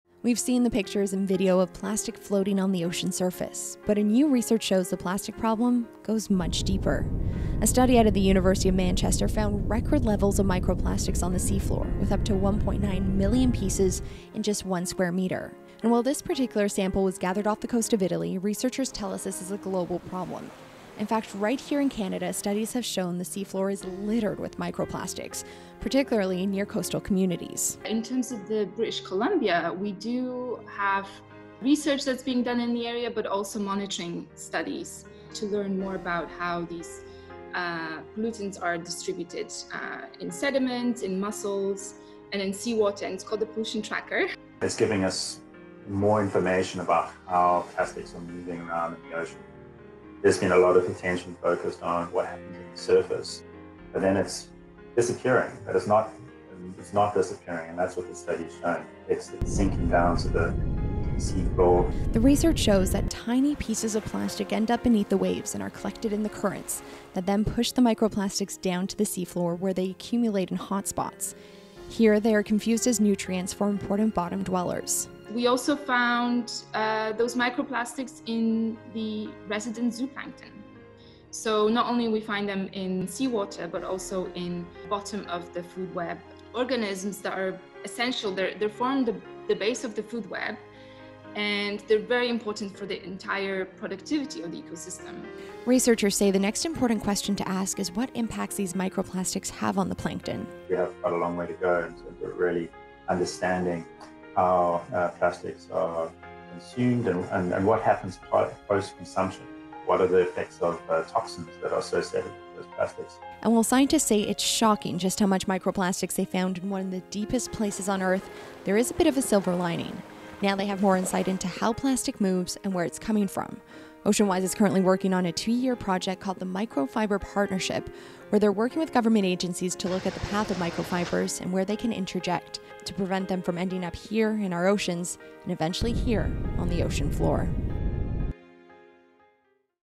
How microplastics are moving throughout our oceans
Researchers have been tracking microplastics and how they collect on the seafloor and start to move through the food chain. Mia Gordon has more.